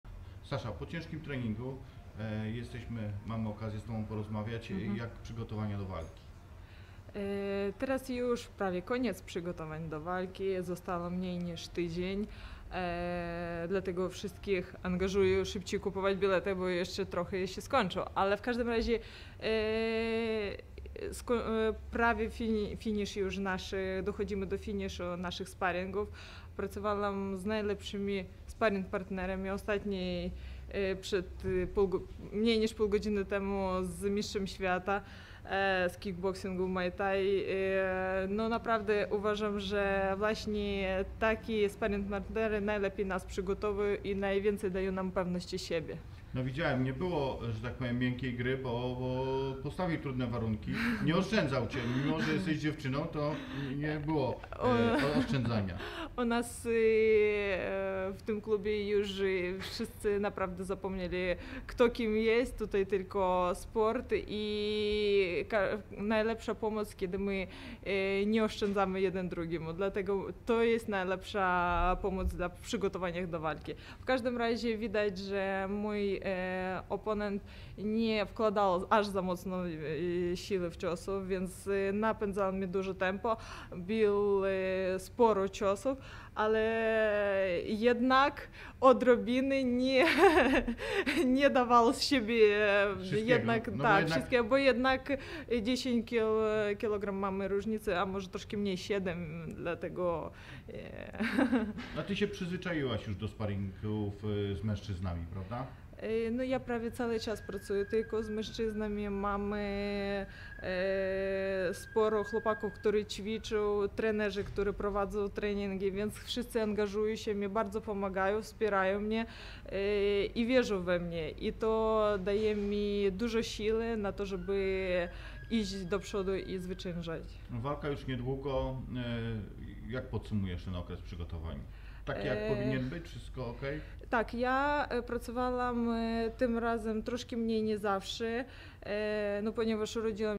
Alexandra Sidorenko: Moim celem jest pas mistrzyni świata
Portal o wszystkich sportach walki przedstawia wywiad z Alexandrą Sidorenko